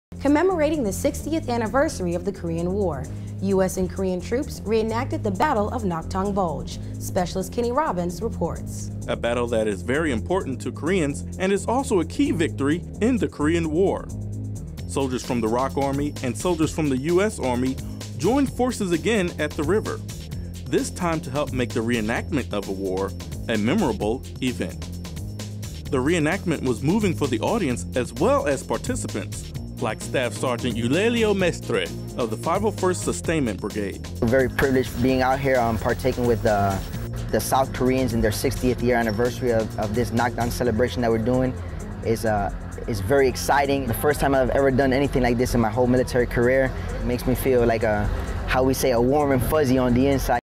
Reenacting the Battle of Naktong Bulge
U.S. and Republic of Korean Soldiers reenact the Battle of Naktong Bulge, commemorating the 60th Anniversary of the Korean War.